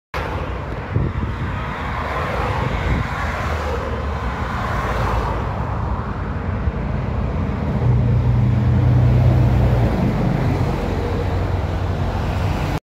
MAN ND313 Lion's City #3113 - linia 194 - Open'er Festival
MAN ND313 Lion's City #3113
Linia: 194
Brygada: 14
Kierunek: Plac Kaszubski

01 lipca 2022